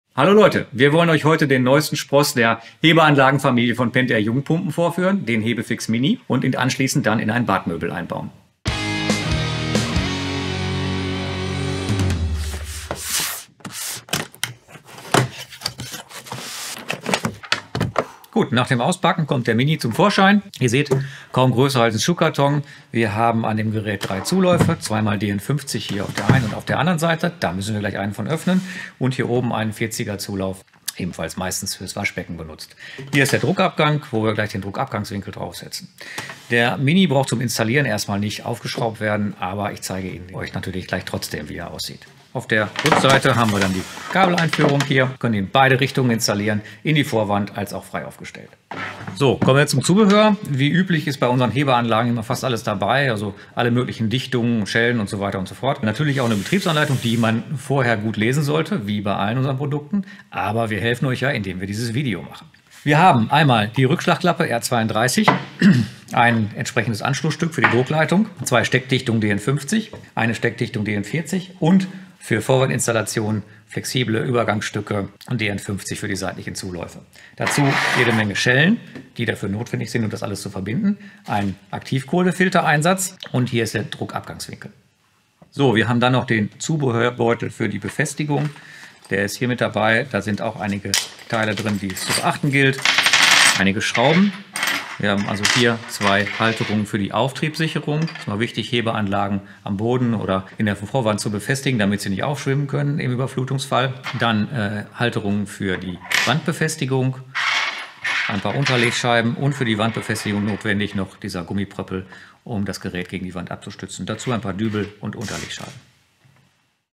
Unboxing HEBEFIX MINI von Jung Pumpen | Kleine Hebeanlage für Schmutzwasser [Part 1/2]
Der Hebefix Mini ist unsere kleinste Schmutzwasser Hebeanlage. Unser technischer Trainer und Abwasser-Experte Jens packt die kleine Hebeanlage aus und erklärt, welches Zubehör sich im Lieferumfang des Hebefix Mini befindet.

Wie man sieht, ist die kleine Schmutzwasserhebeanlage kaum größer als ein Schuhkarton. Wollt ihr mehr Unboxing Videos zu unseren Produkten, wie Schmutzwasserpumpen, Hebeanlagen oder Tauchpumpen? Dann schreibt uns eure Vorschläge gerne in die Kommentare.

Jung Pumpen GmbH ⇨ ✓ ☎ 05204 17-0